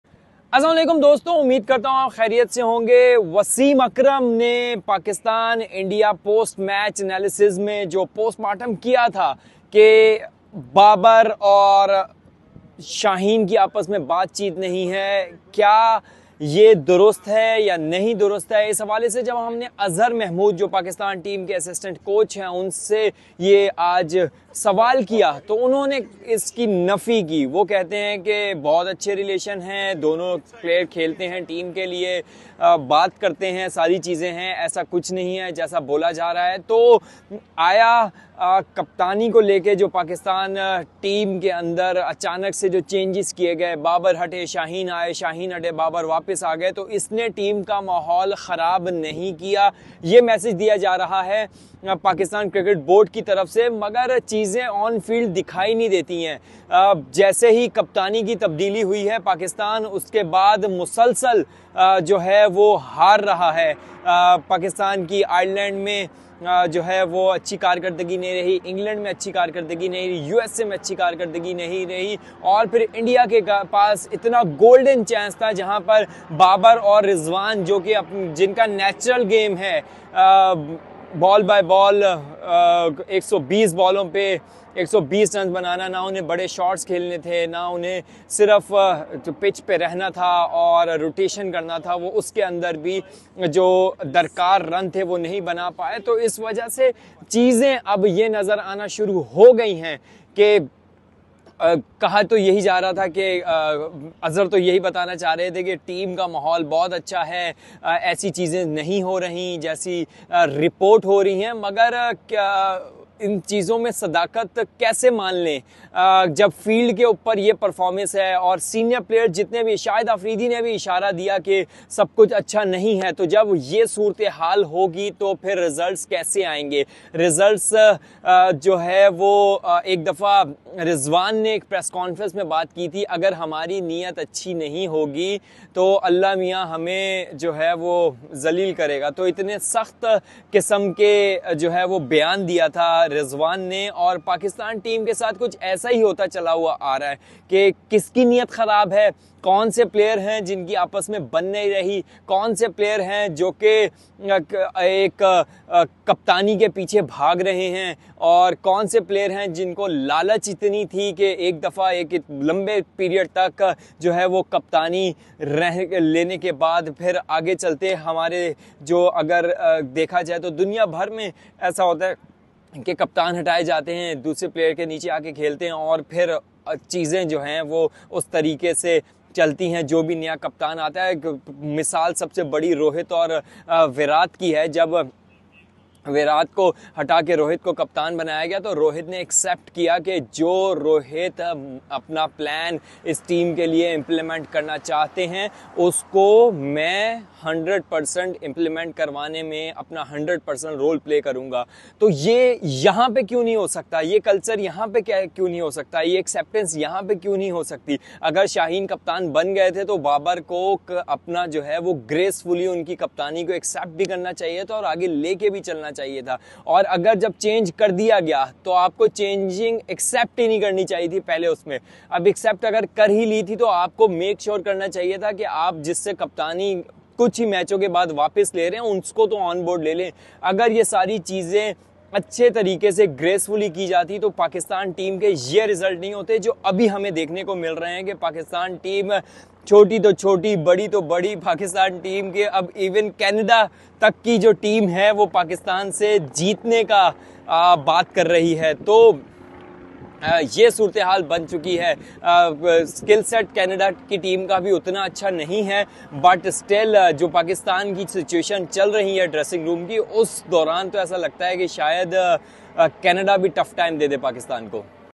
Babar vs. Shaheen? What is happening inside the Pakistani dressing room? | T20 World Cup
Babar vs. Shaheen? What is happening inside the Pakistani dressing room? Here are insights from our correspondent in New York.